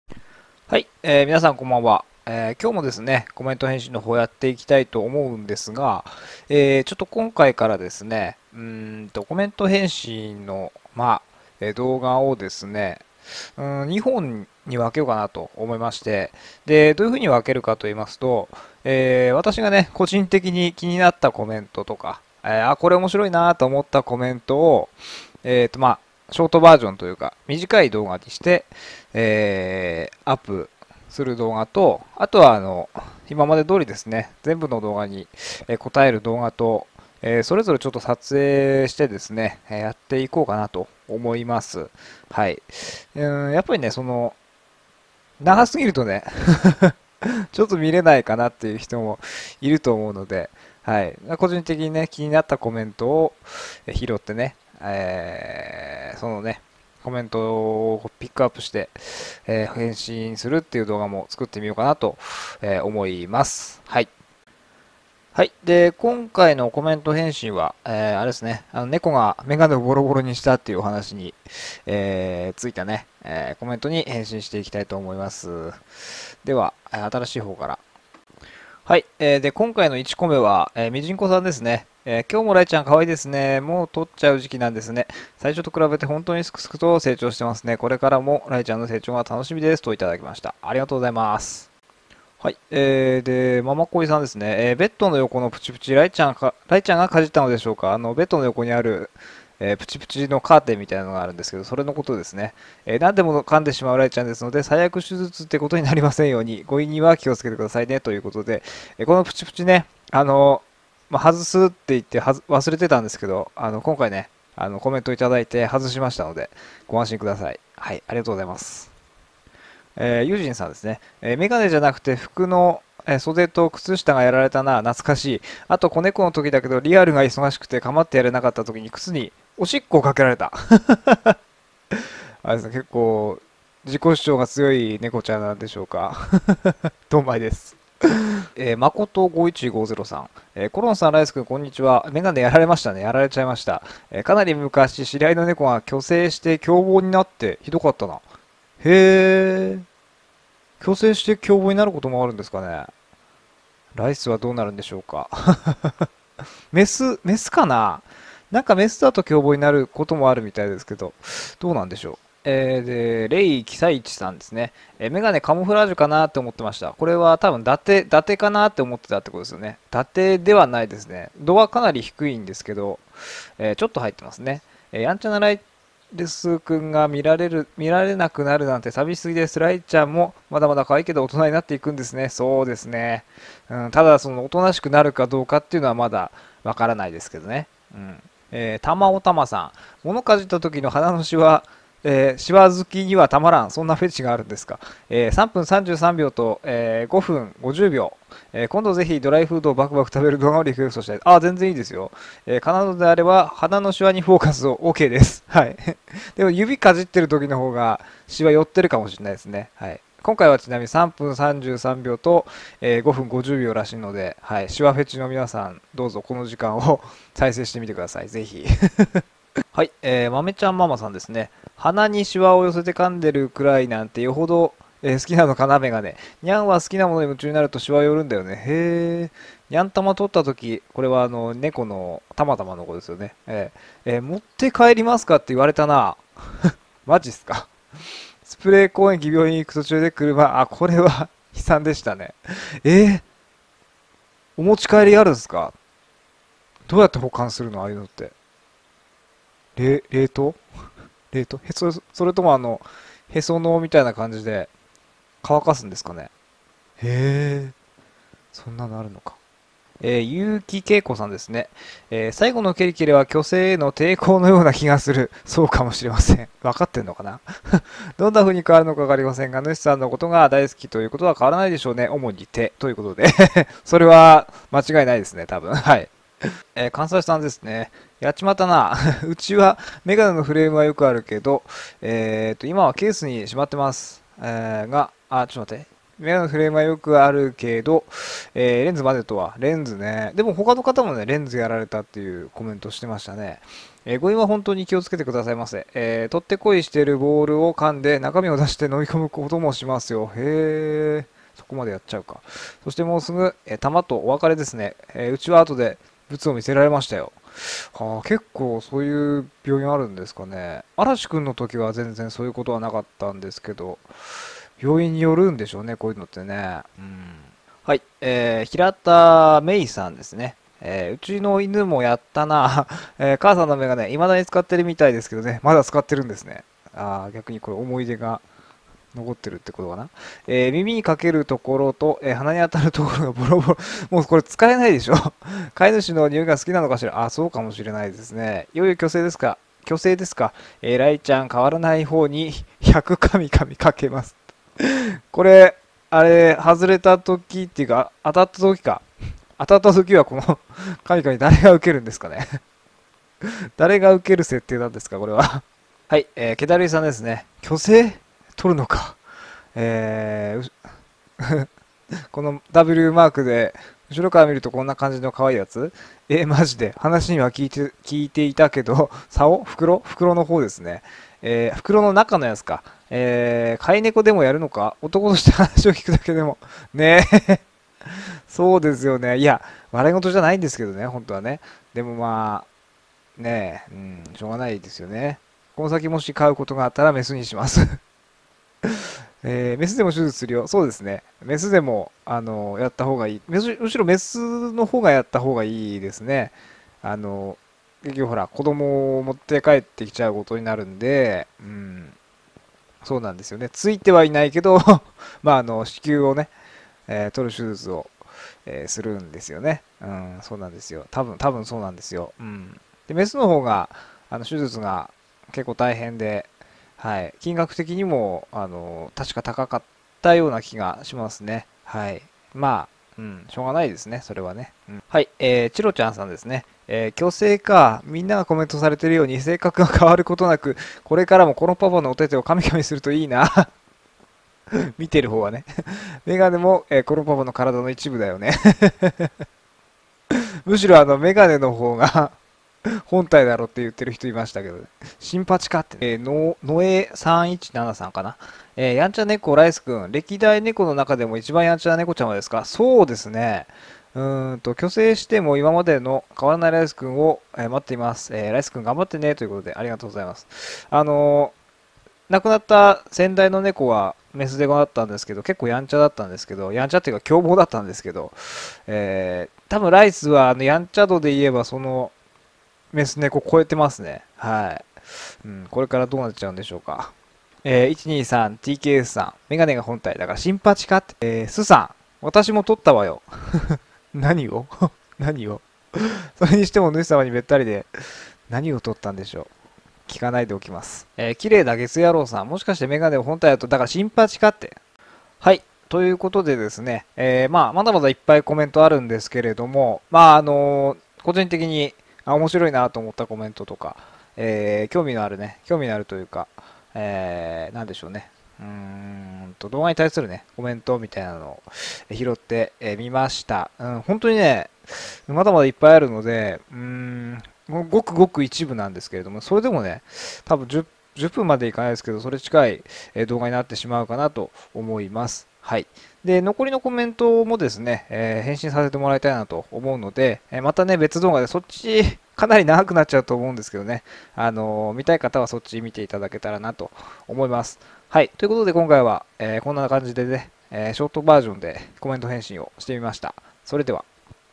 【コメント返信】猫がある物をボロボロにしたお話
今回は気になったコメントを短めにまとめてみました。